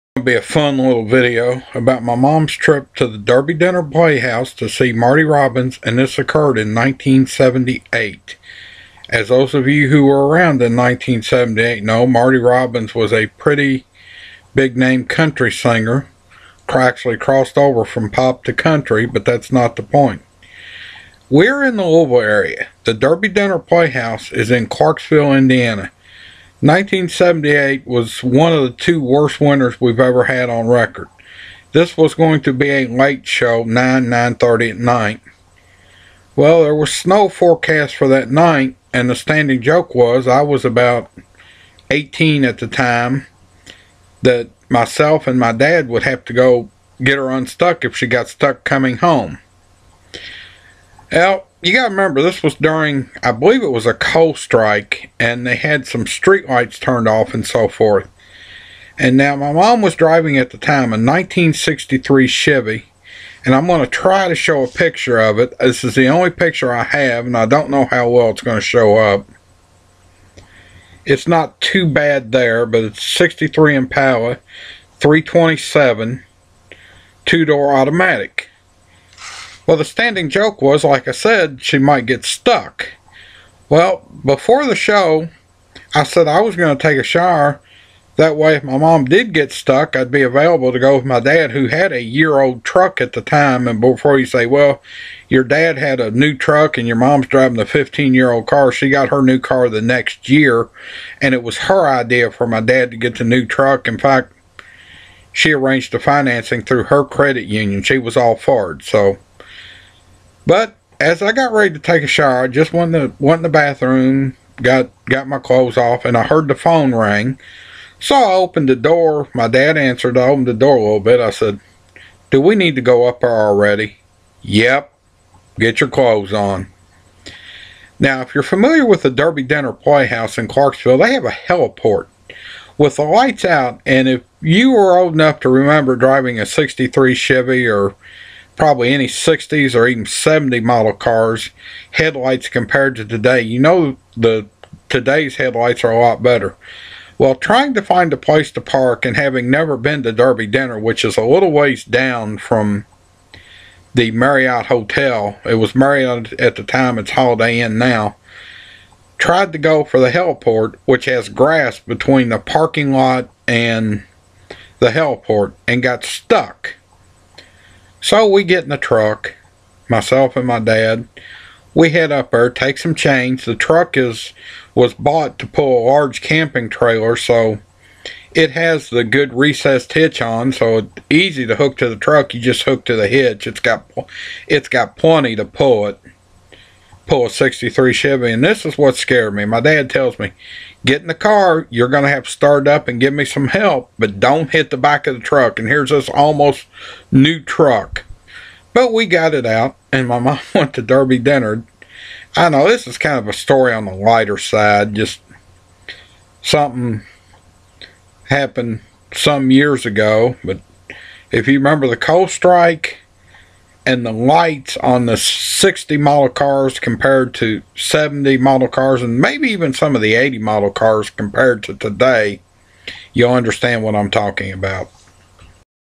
Derby Dinner trip 1978.
This is a fun little story about my Mom's trip to the Derby Dinner Playhouse in 1978. In this vlog I detail the problems of snow and due to a coal strike some street lights being out. I also show a bad picture of the car she was driving and got stuck in as well as the story of my Dad and myself pulling the car out.